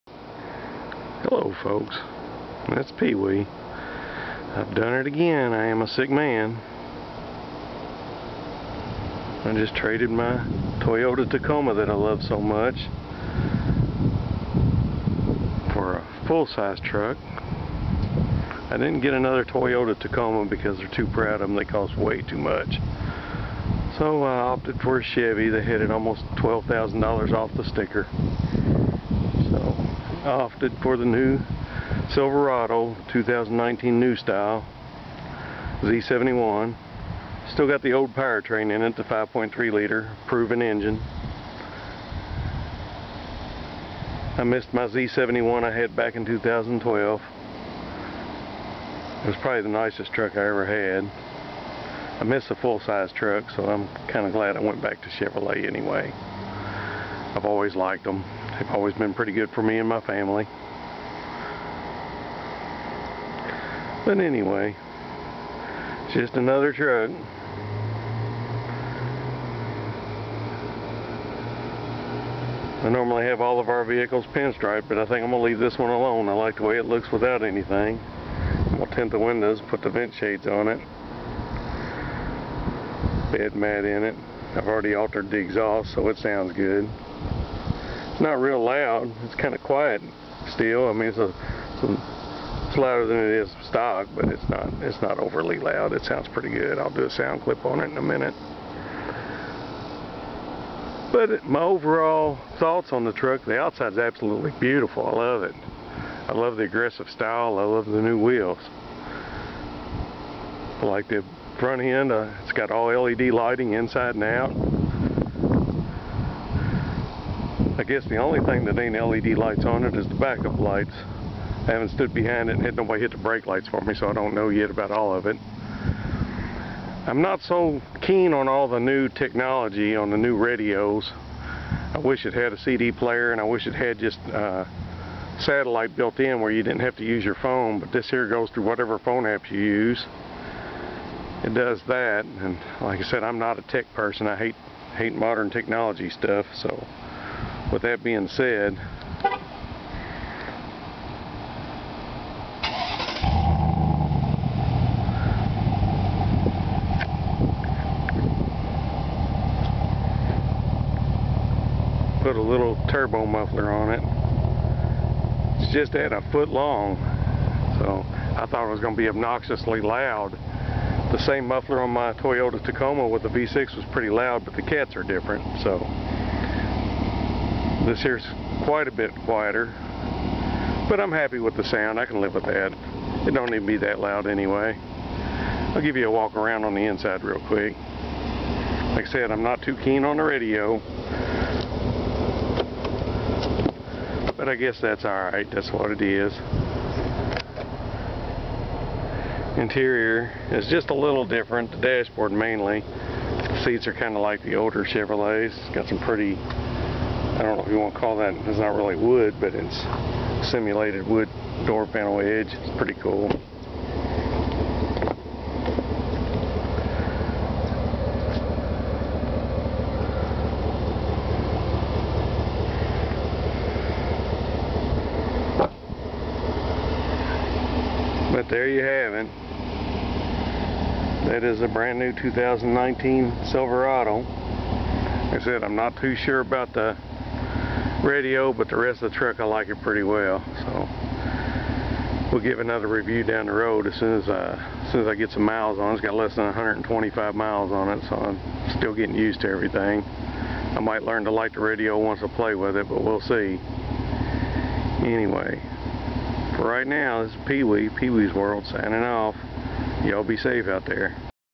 New Silverado Z-71 for 2019
New 2019 Chevrolet Silverado Z-71 RST package , just picked this up last week , I will make a more in depth video at a later date , with real thoughts , and impressions I get from this truck .